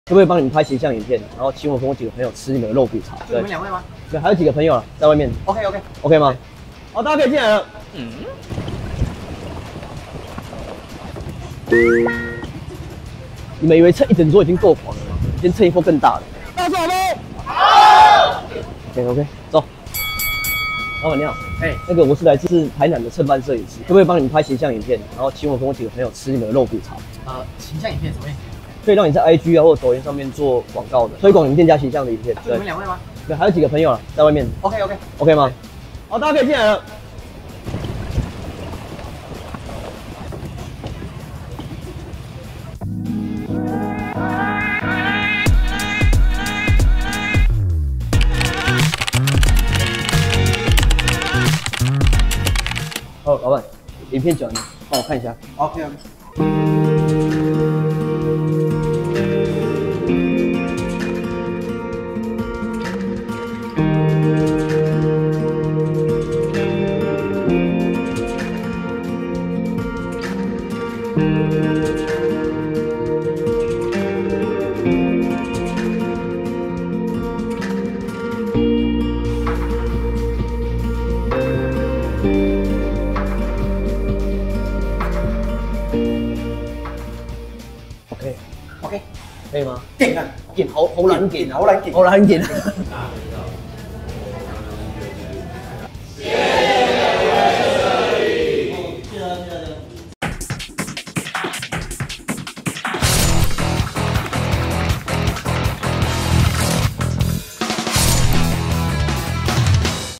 獨家／網紅帶20人吃霸王餐！老闆傻眼…結局竟反轉「全部免費招待」｜三立娛樂星聞
★訂閱【女孩要幹嘛】甜寵浪漫戲劇看到飽